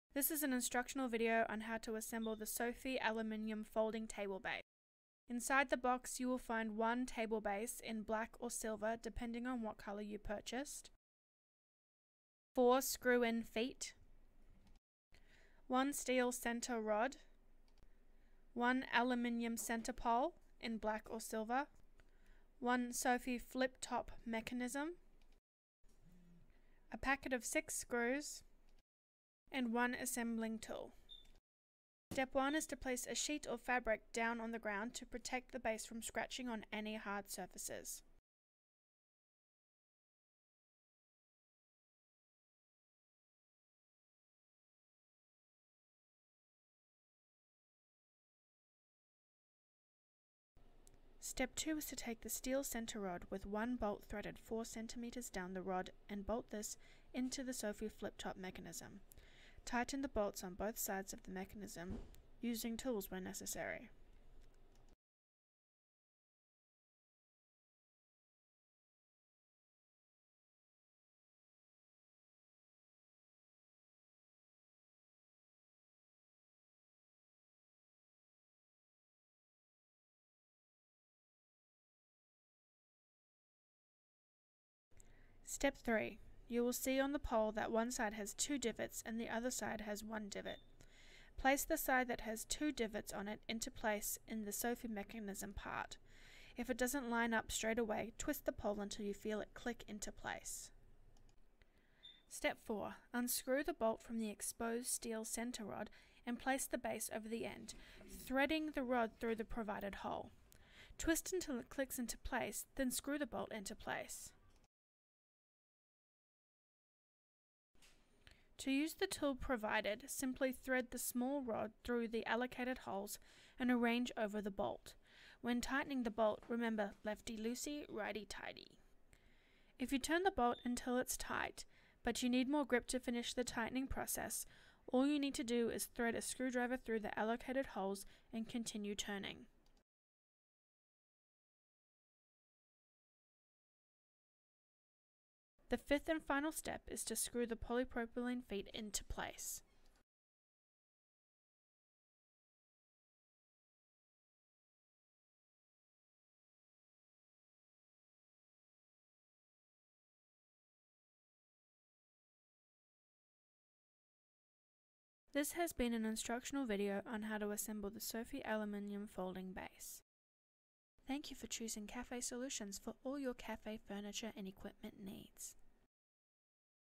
How to assemble the Sophie Aluminium Table Base
This is an instructional video on how to assemble the Sophie AluminumTable Base.